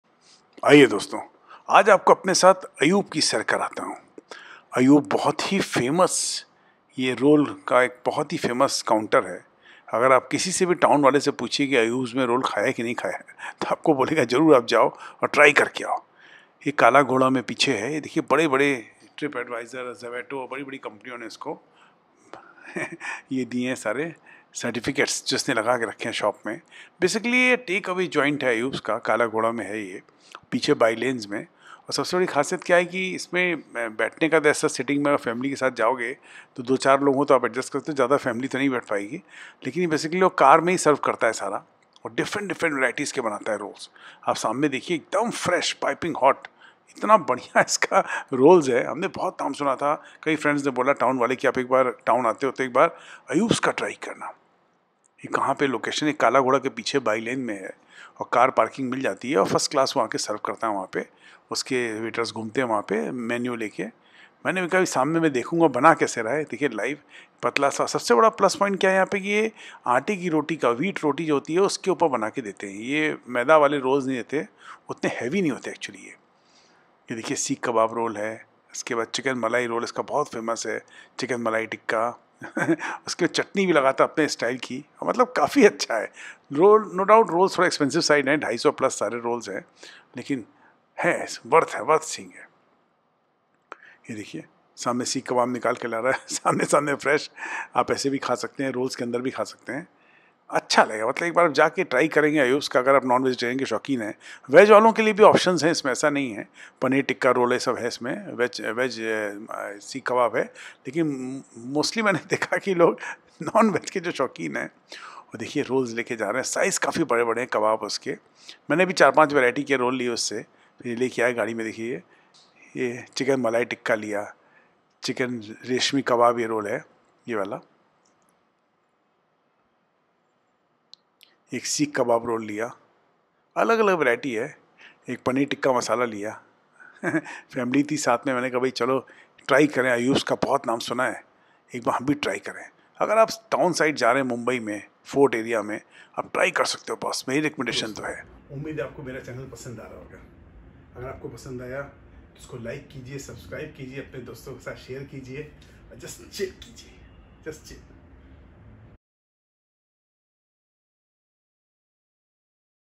Ayub's Rolls- Bollywood celebrities favourite Rolls Joint ll Mumbai's Famous Rolls at Ayub's ll
Step into the glitz and glam of Bollywood at Ayub's Rolls, Colaba! 🎬 Savored by your favorite stars and now endorsed by Juzz Chill! 🌟 Indulge in the legendary flavors that keep the celebs coming back for more! 🥙✨